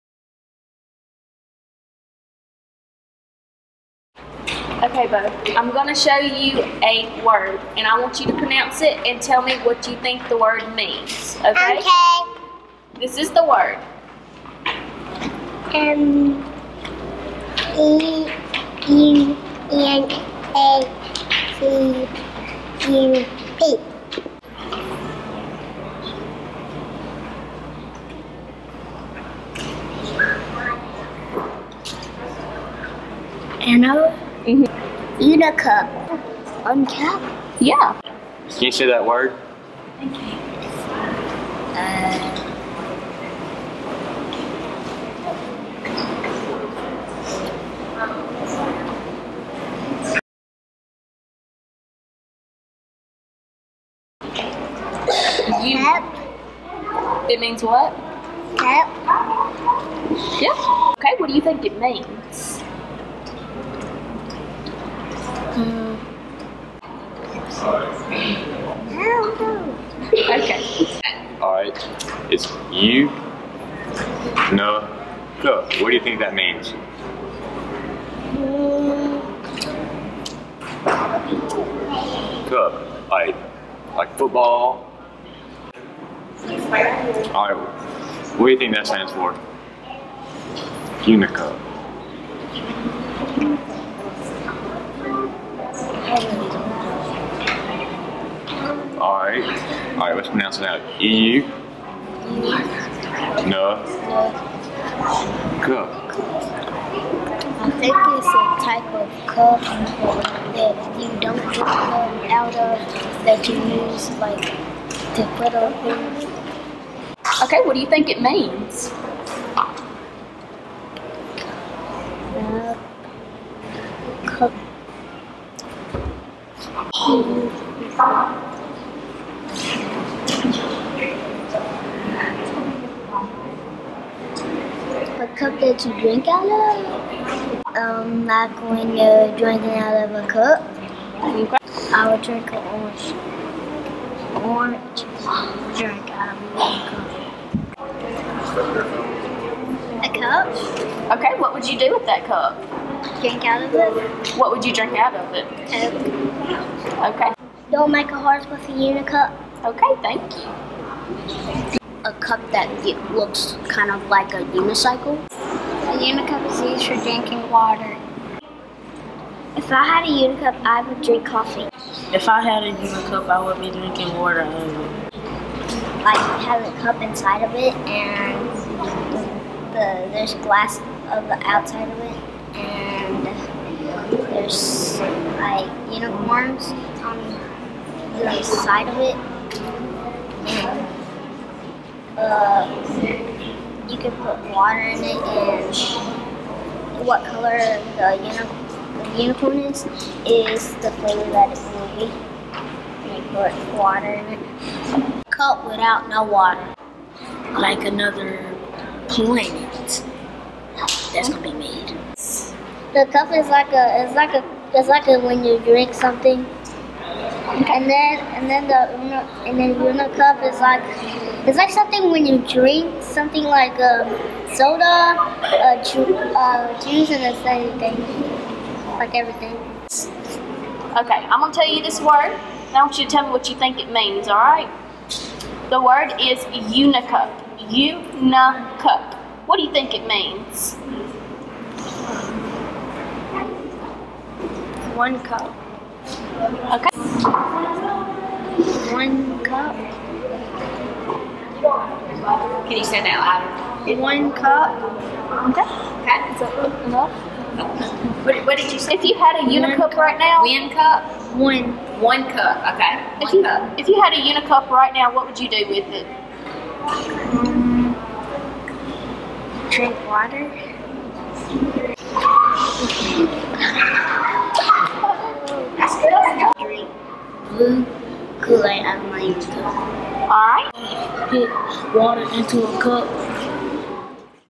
EUNA Cup
The Elementary Students try to pronounce the word EUNA Cup and tries to figure out what it means in preparations for the first ever rivalry against the New Albany Bulldogs And The East Union Urchins.